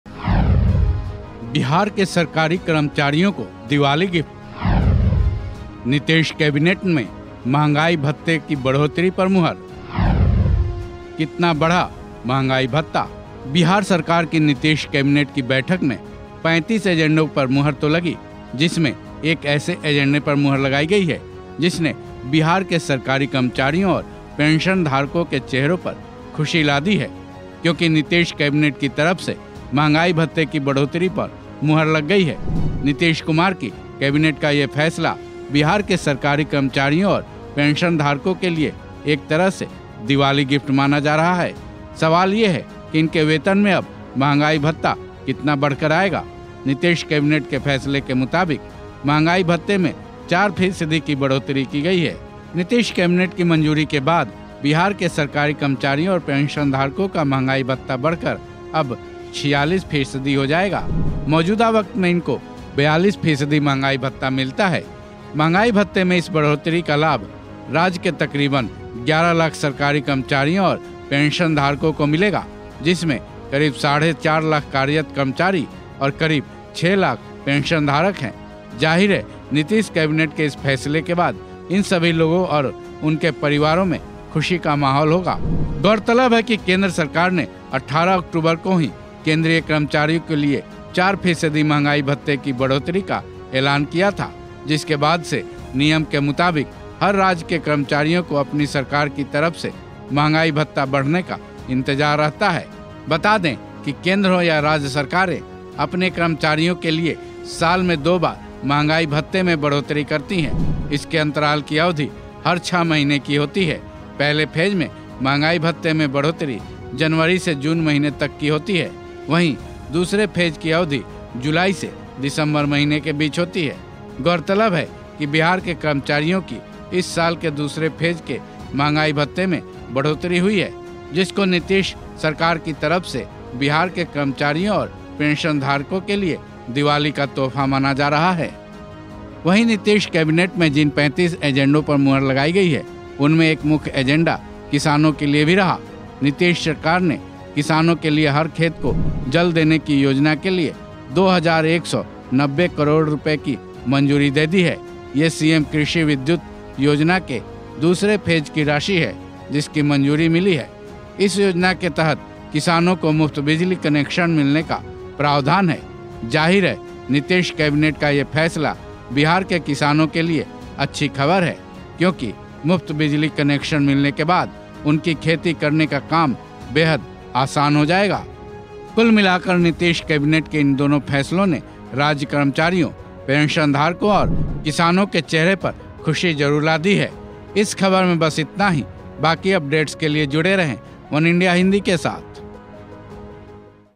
7th Pay Commission : Nitish Kumar ने कर्मचारियों का कितना बढ़ाया Dearness Allowance | वनइंडिया हिंदी
Bihar Cabinet Meeting: नीतीश कुमार (Nitish Kumar ) की सरकार ने कैबिनेट मीटिंग (Cabinet Meeting) में एक बड़ा फैसला लिया है.  इस कैबिनेट की बैठक मे लिया गया फैसला बिहार के सरकारी कर्मचारियों (Government Employees of Bihar) और पेंशनधारकों (pensioners) के लिए दिवाली का तोहफा(Diwali Gift)साबित हुआ है.  बिहार सरकार (Bihar Government) की कैबिनेट की बैठक में महंगाई भत्ते (Dearness Allowence) की बढ़ोतरी पर मुहर लगा दी गई है.  जिसके बाद अब सैलरी बढ़कर आएगी.  नीतीश कुमार की कैबिनेट (Nitish Kumar Cabinet) ने कितना महंगाई भत्ता बढ़ाया है ये जानने की उत्सुकता बिहार के कर्मचारियों में दिख रही है.